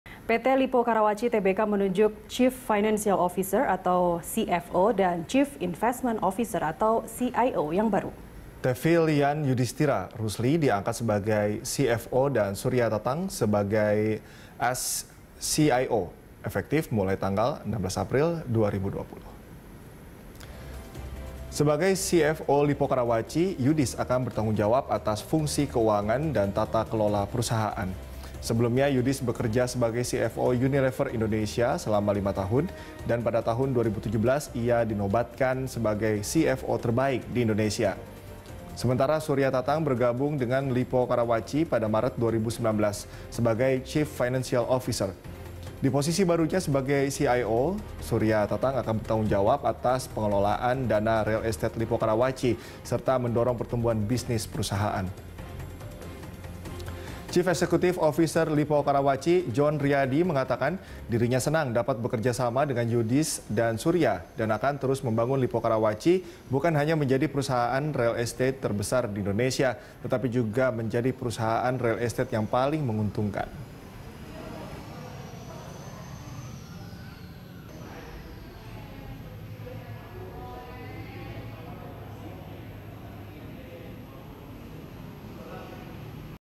Lippo Karawaci Tunjuk CFO dan CIO Baru
PT Lippo Karawaci Tbk menunjuk Chief Financial Officer (CFO) dan Chief Investment Officer (CIO) baru Tevilyan Yudhistira Rusli diangkat sebagai CFO dan Surya Tatang sebagai AS CIO efektif mulai tanggal 16 April 2020.